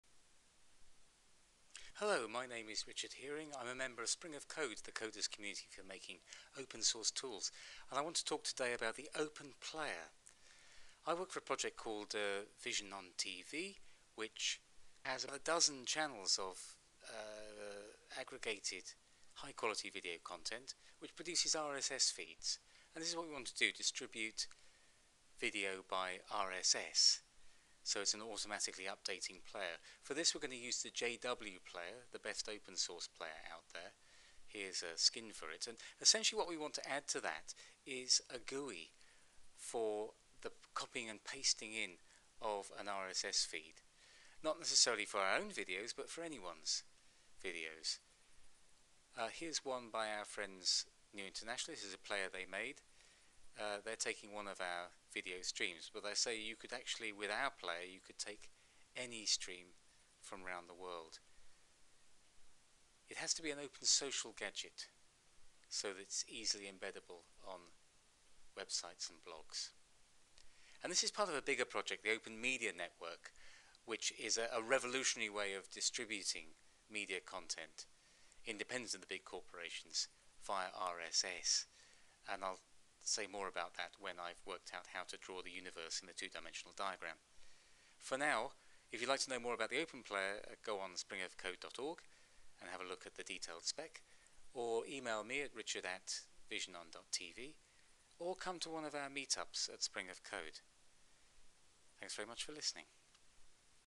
Introducing the OpenPlayer
The OpenPlayer enables you to watch videos from your favourite source on your own site or blog. As it takes any rss feed you put in, it creates an automatically-updating channel of video content. Part of visionOntv's Open Media Network project at Spring of Code.
Introduction by Richard Hering.